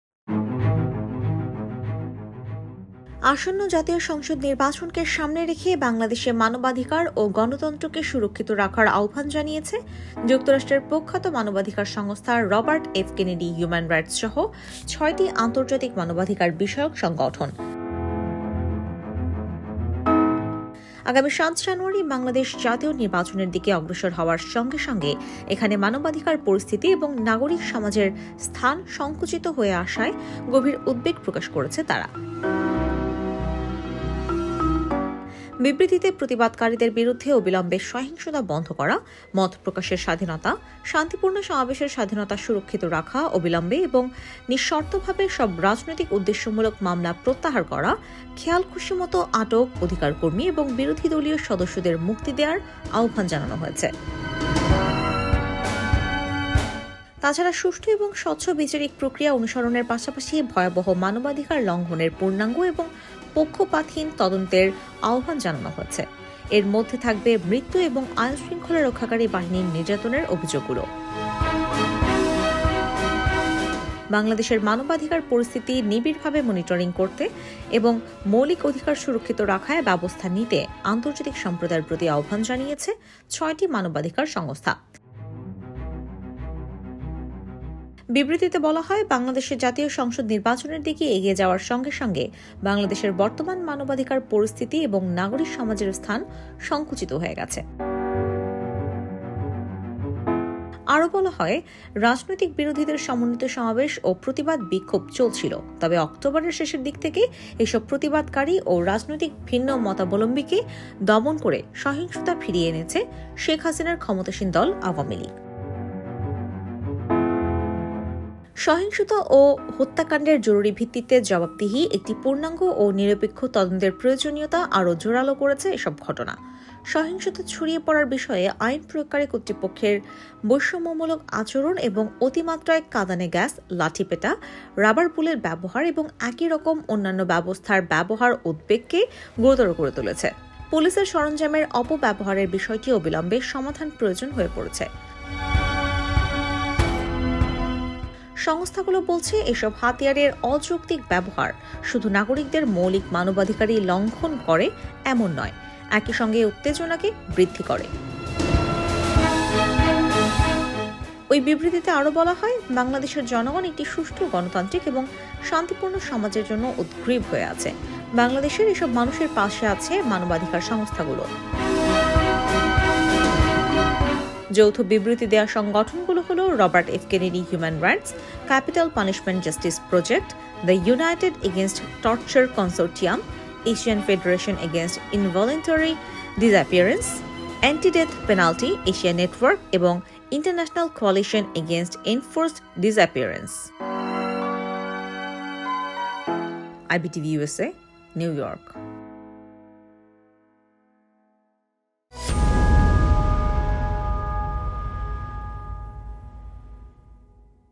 বিএনপিসহ বিরোধীদের পাশে থাকবে মানবাধিকার সংস্থাগুলো | Election | Politics | Bangladesh
Human rights organizations will stand by the opposition including BNP